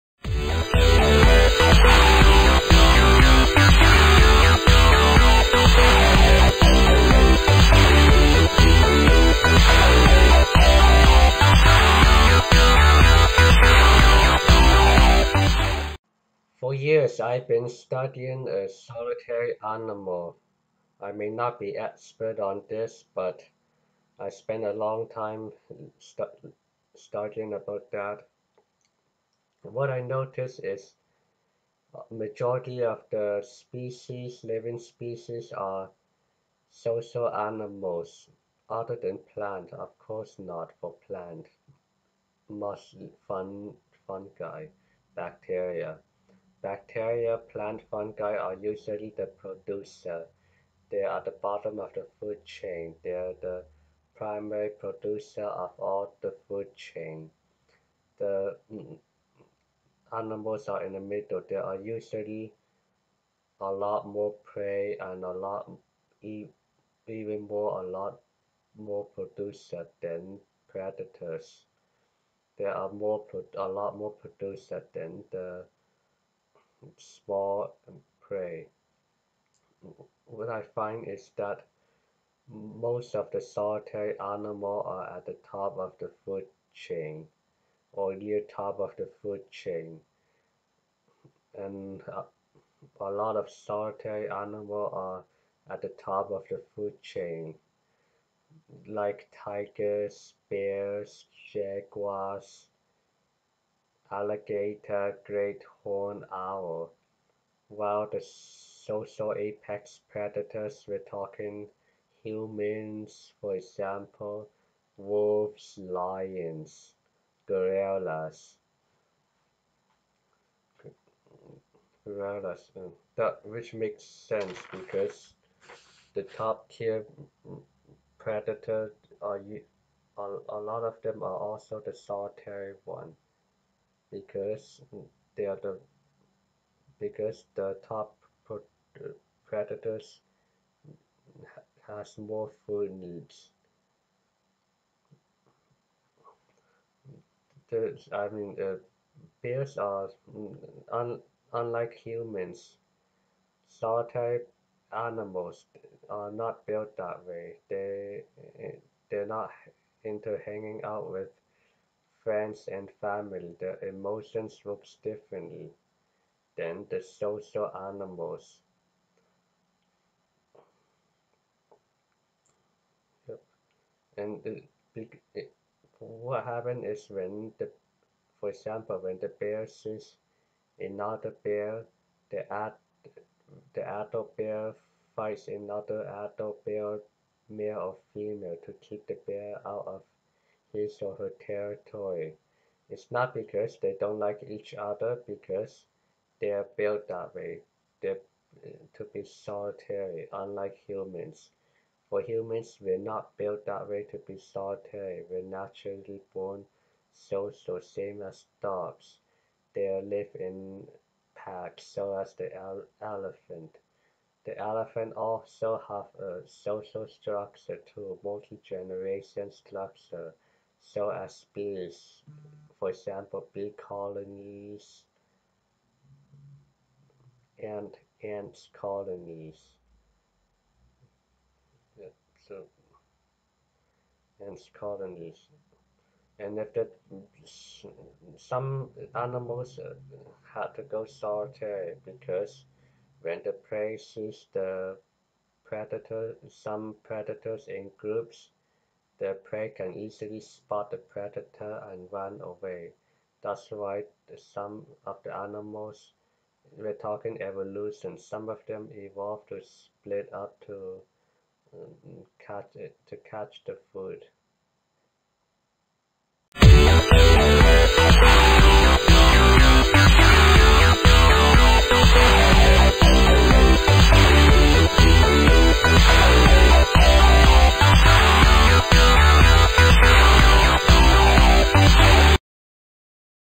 Talk about Solitary Animals
Most or almost all solitary animals are at the top or near the top of the food chain, which makes sense because there is usually a lot more prey even more producers than predators. More prey means enough food for predators, according to trophic levels.